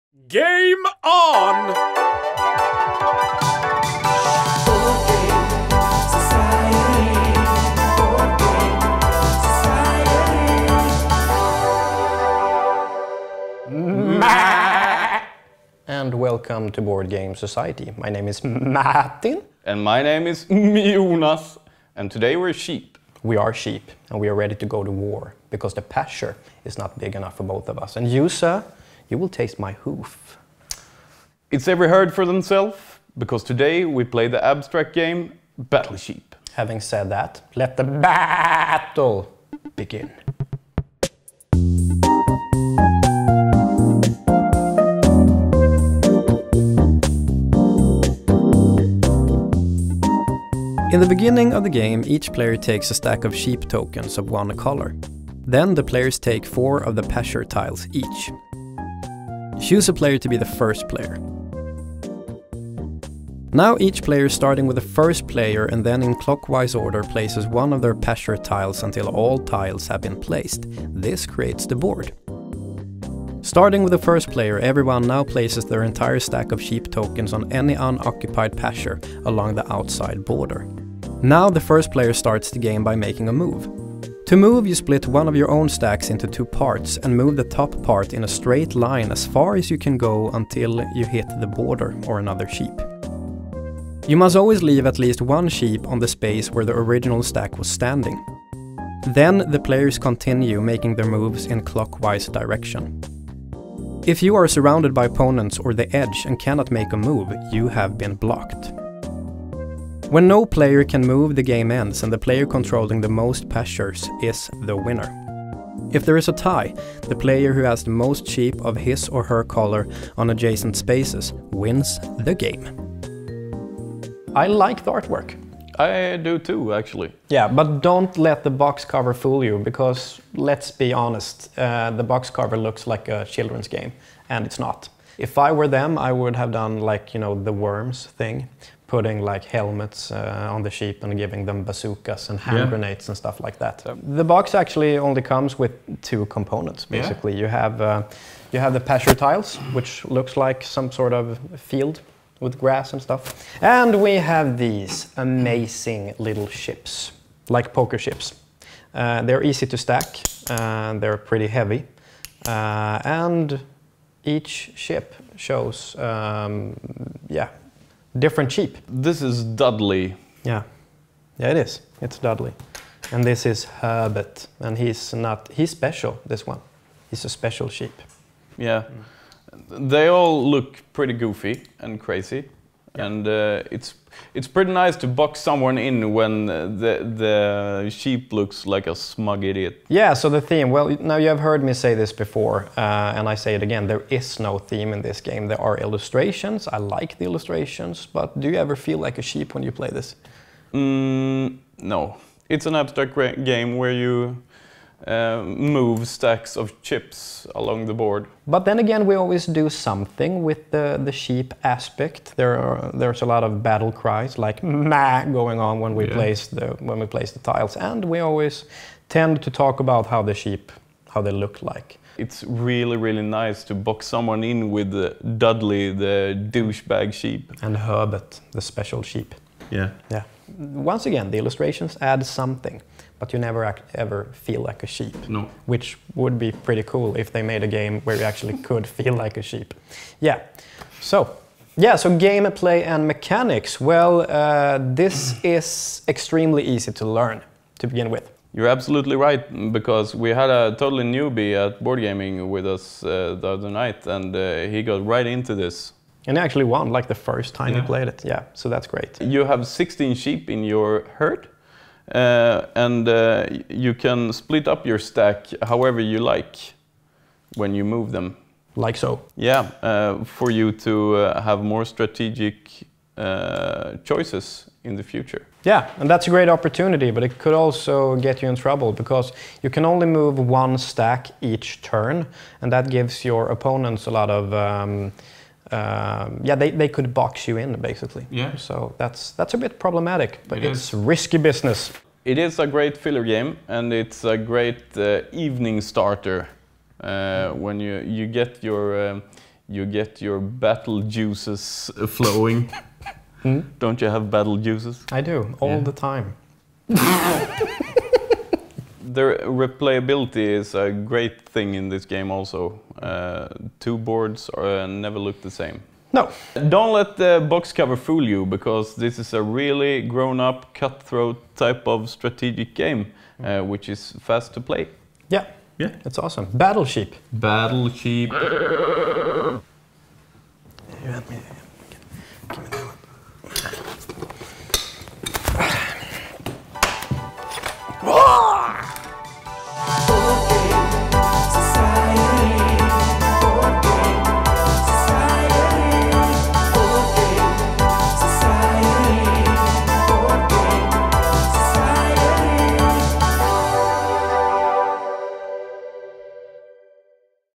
BGS - Episode 10 - Battle Sheep review
Martin and Jonas goes to war. With sheep. This week we take a look at the abstract game Battle Sheep. Let the baaaattle begin!

Designers: Francesco Rotta
Publishers: Blue Orange Games & Lautapelit.fi